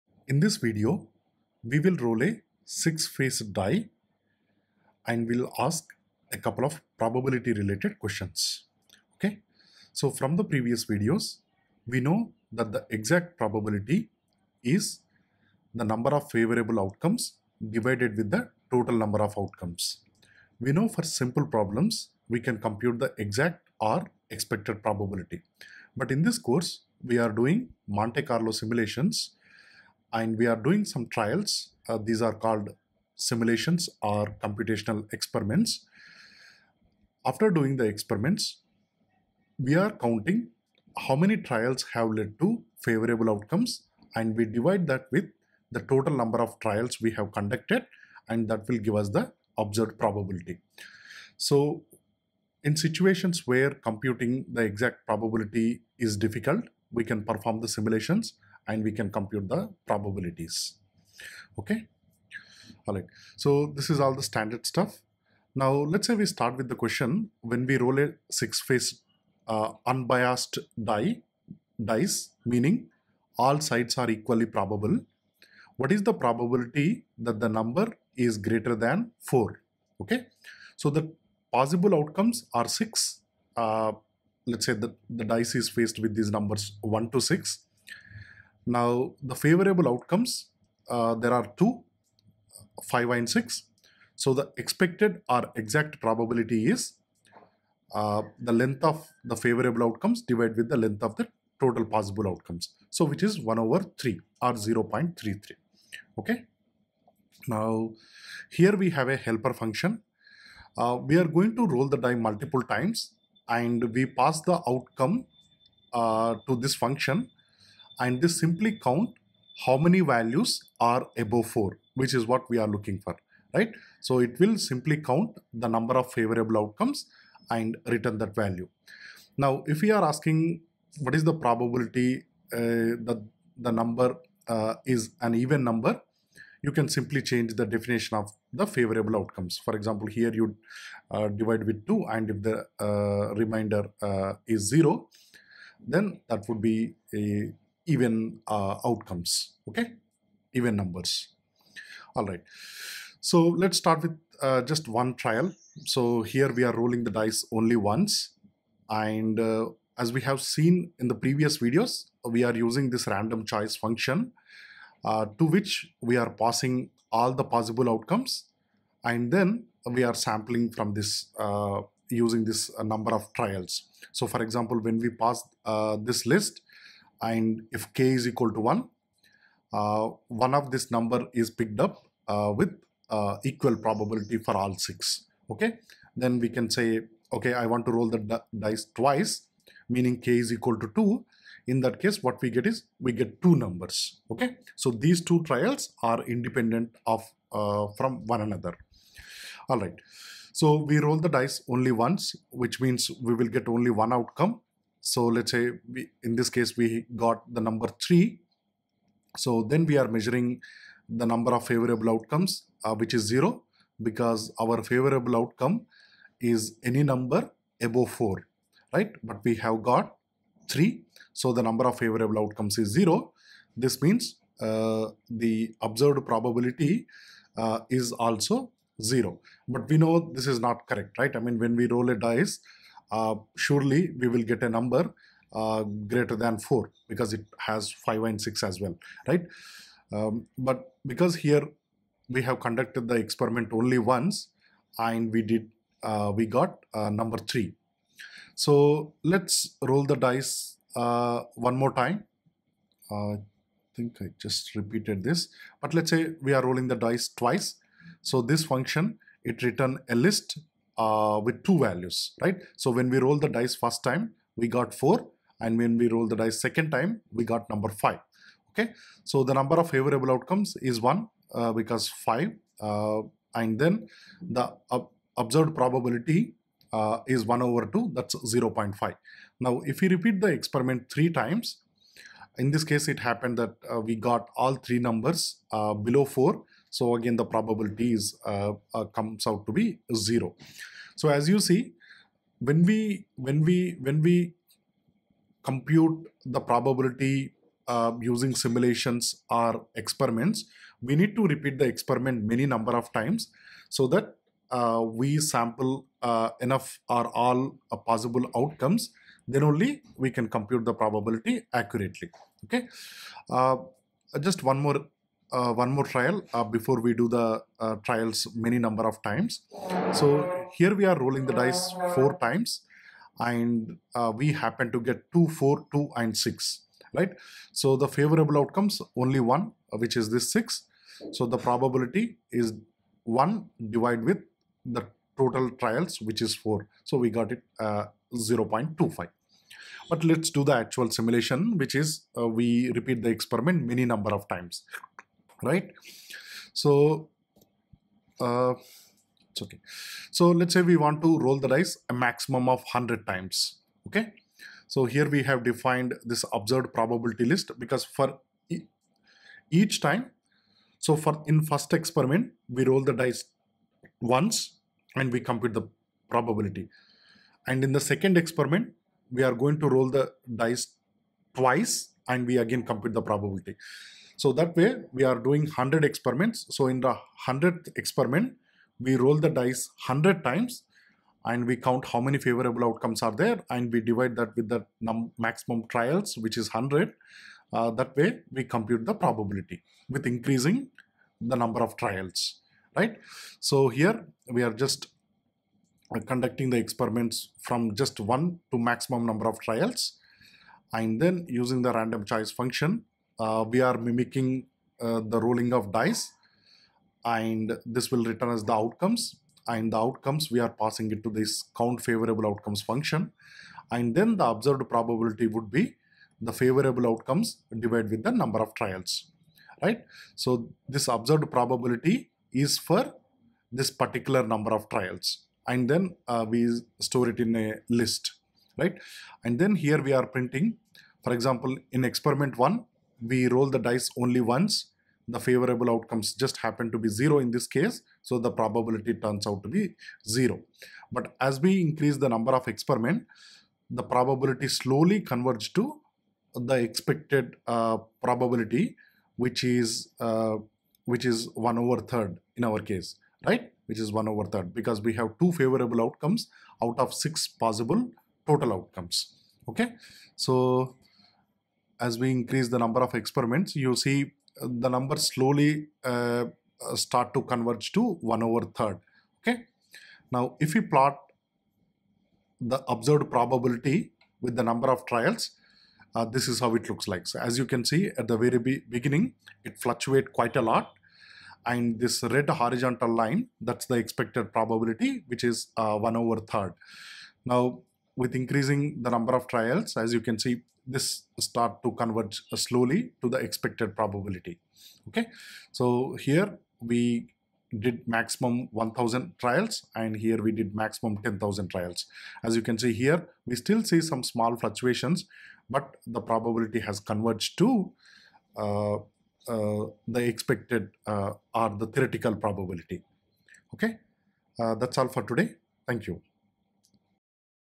Monte Carlo Simulations - Roll a Dice
Monte Carlo Simulations

- Probablity
- Random numbers
- Sampling techniques
- Probability distributions
- Law of large numbers
- Experimental/Simulation design